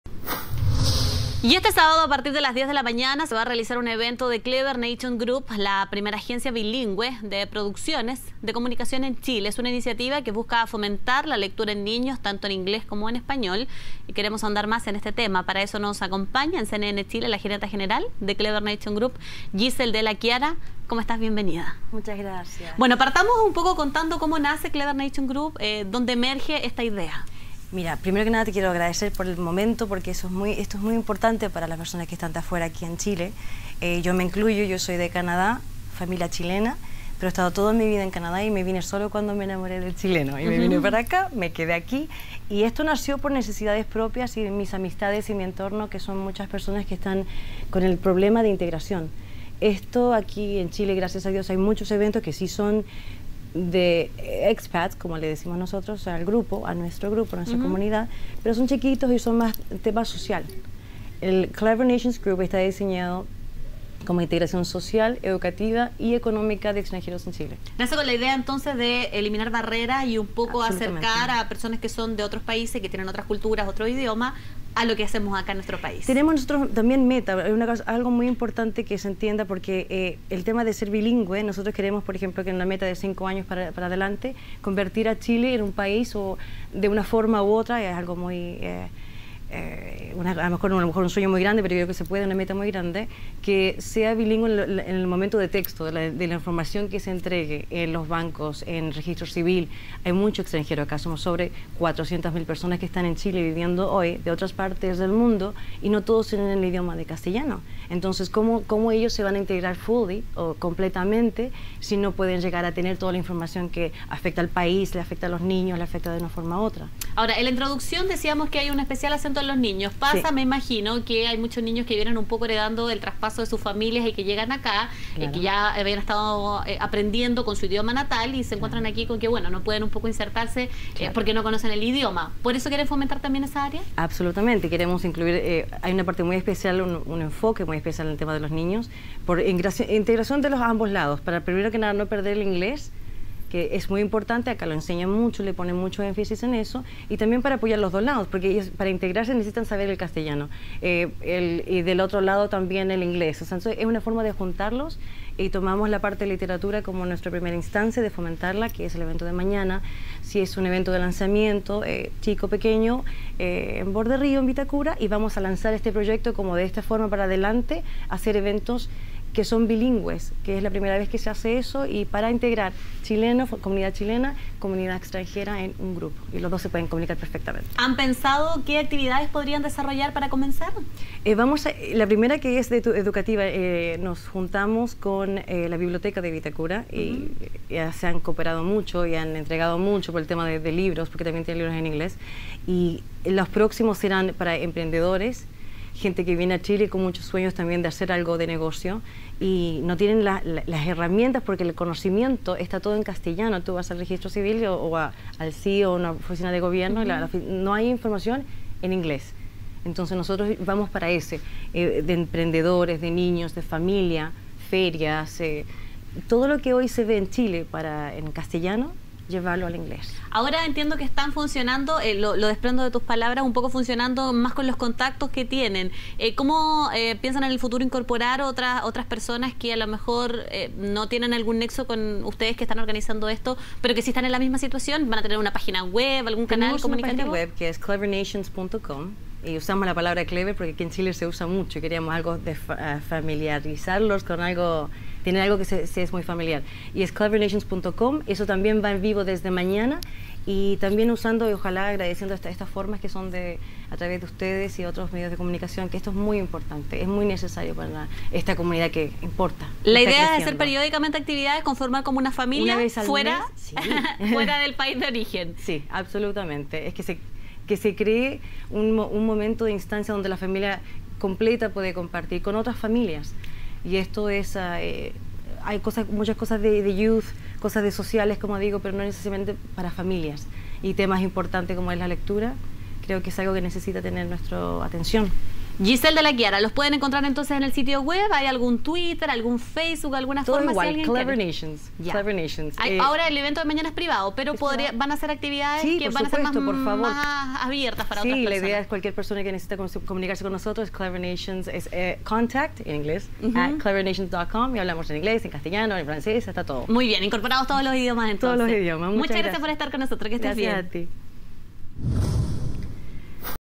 Gisel Della Chiara profundizó respecto del primer CleverNations Group en Chile
Idea emerge como forma de acotar la brecha del lenguaje y su efecto excluyente.